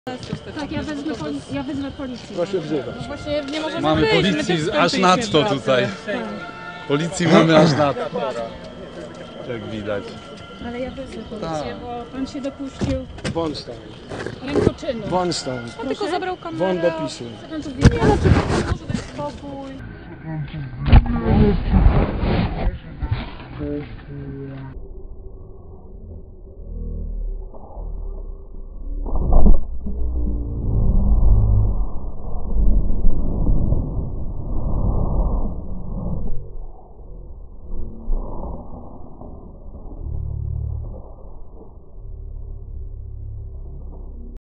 Pełnosprawny atak na sprzęt - akcja posła Jacka Kwiatkowskiego
Niepełnosprawny Poseł, okazał się bardzo sprawny, atakując sprzęt dziennikarki. Poseł Jacek Kwiatkowski z ruchu Palikota pochodzi z miasta Konin.